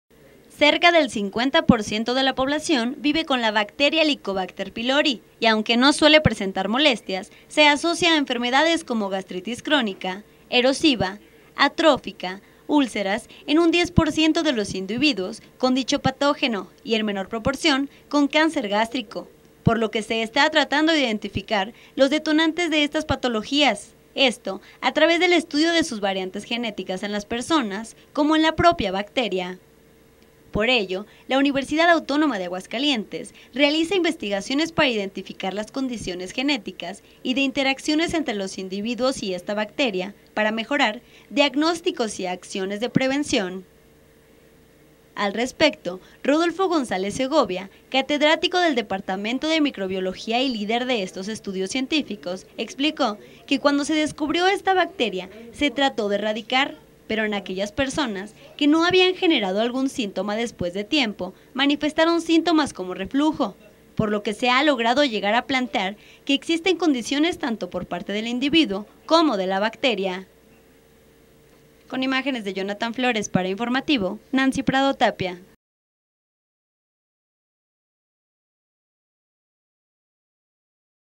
EN LA UAA SE ANALIZAN VARIANTES DE LA BACTERIA HELICOBACTER PYLORI
Cerca del 50 por ciento de la población vive con la bacteria Helicobacter pylori.

Reportera: Nancy Prado Tapia
Camarógrafo: Jonathan Flores